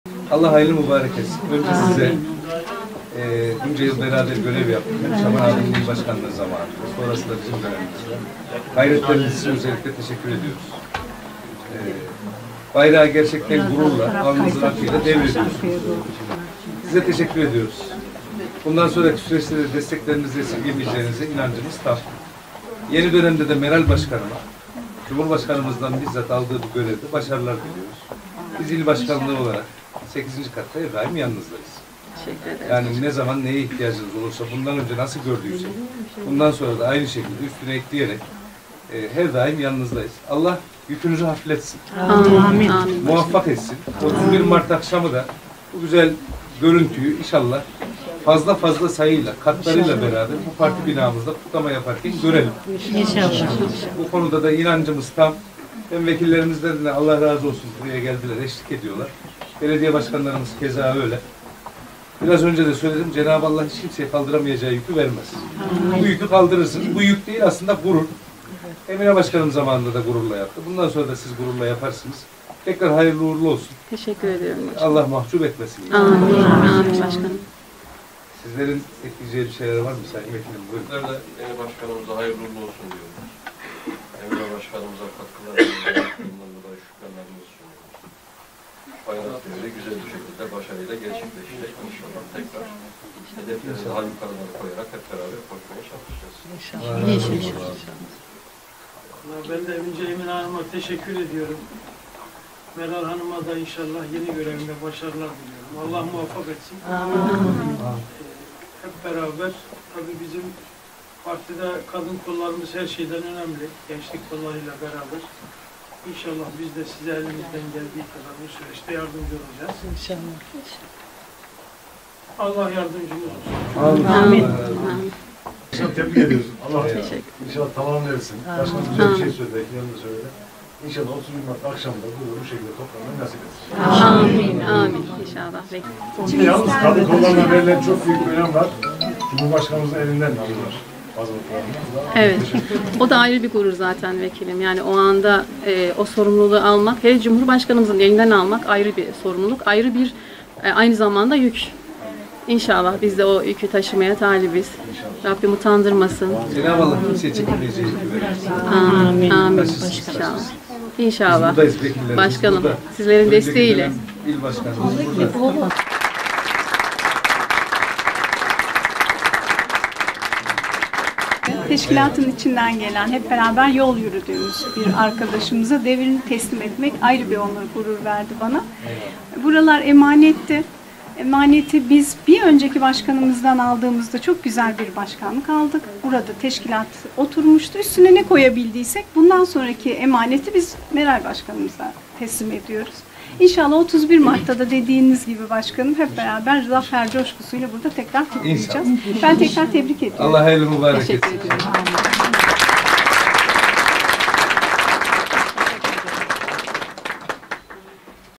AK Parti Kayseri İl Kadın Kollarında Bayrak Değişimi
AK Parti Kayseri İl Kadın Kollarında; AK Parti Kayseri İl Başkanı Fatih Üzüm, AK Parti Kayseri Milletvekilleri Şaban Çopuroğlu, Murat Cahid Cıngı, Sayın Bayar Özsoy ve AK Parti Kayseri teşkilat mensuplarının katılımıyla İl Kadın Kolları Başkanlığı devir teslim töreni gerçekleşti. Önceki Dönem AK Parti Kayseri İl Kadın Kolları Başkanı Emine Timuçin, İl Kadın Kolları Başkanı olarak seçilen Meral Koşar’a görevini teslim etti.